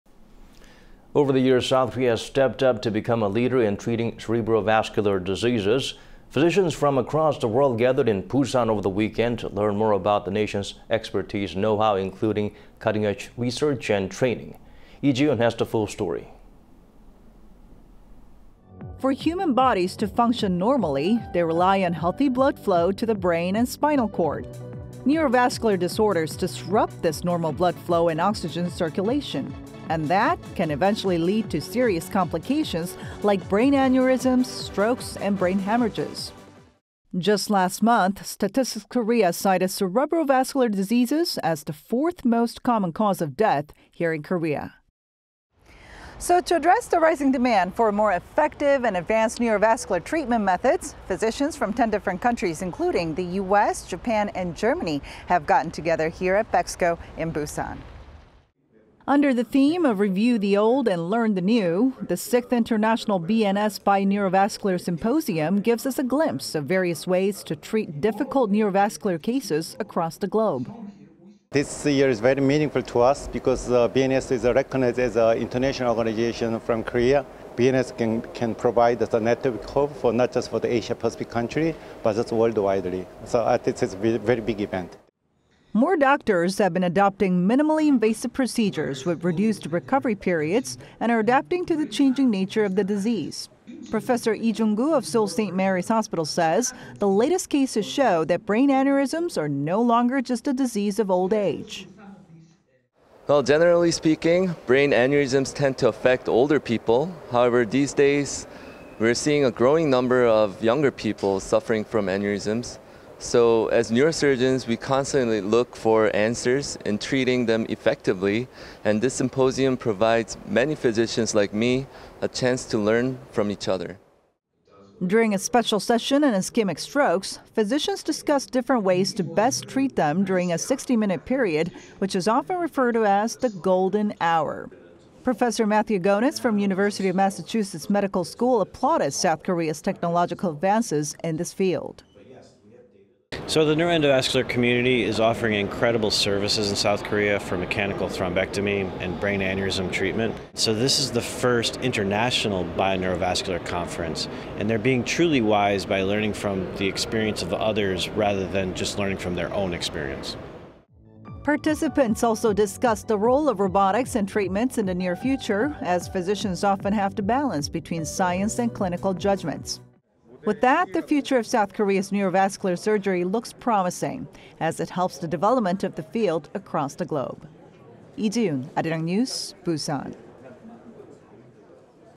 Promoting South Korea's neuroendovascular treatment methods on global stage '6th Int'l BNS ...
Promoting South Korea's neuroendovascular treatment methods on global stage '6th Int'l BNS conference'

한국의 뇌혈관 의학의 세계화 '제6회 국제BNS뇌혈관심포지엄'

Over the years, South Korea has stepped up to become a leader in treating cerebro-vascular diseases.
Physicians from across the world gathered in Busan over the weekend to learn more about the nation's know-how including cutting-edge research and training
Lee Ji-yoon has the full story.

For human bodies to function normally, they rely on healthy blood flow to the brain and spinal cord.
Neurovascular disorders disrupt this normal blood flow and oxygen circulation.
And THAT can eventually lead to serious complications like brain aneurysms, strokes and brain hemorrhages.
Just last month, Statistics Korea cited cerebrovascular diseases as the fourth most common cause of death here in Korea.

"So to address the rising demand for more effective and advanced neurovascular treatment methods physicians from ten different countries including the U.S., Japan and Germany have gotten together here at BEXCO in Busan."

Under the theme of "review the old and learn the new," the Sixth International BNS Bi-Neurovascular Symposium gives us a glimpse of various ways to treat difficult neurovascular cases across the globe.

"This year very meaningful to us because BNS (Bi-Neurovascular Symposium) has been formally recognized as an international organization in Korea. BNS can provide a network hub for not only Asian Pacific countries but for the world, so this is a very big event."

More doctors have been adopting minimally invasive procedures with reduced recovery periods and are adapting to the changing nature of the disease.
Professor Lee Jung-koo of Seoul St. Mary's Hospital says, the latest cases show that brain aneurysms are no longer just a disease of old age.

"Generally speaking, brain aneurysms tend to affect older people. However, these days, we are seeing a growing number of younger people suffering from aneurysms. So as neurosurgeons we constantly look for answers in treating them effectively and this symposium provides many physicians like me a chance to learn from each other."

During a special session on ischemic strokes physicians discussed different ways to best treat them during a 60-minute period which is often referred to as the "golden hour."
Professor Matthew Gounis from University of Massachusetts Medical School applauded South Korea's technological advances in this field.

"So the neuroendovascular community is offering incredible services in South Korea from mechanical thrombectomy and brain aneurysm treatment. So this is the first international bi neurovascular conference and they're being truly wise by learning from experience of others rather from their own experience."

Participants also discussed the role of robotics in treatments in the near future as physicians often have to balance between science and clinical judgements.
With that, the future of South Korea's neurovascular surgery looks promising as it helps the development of the field across the globe
Lee Ji-yoon, Arirang News, Busan.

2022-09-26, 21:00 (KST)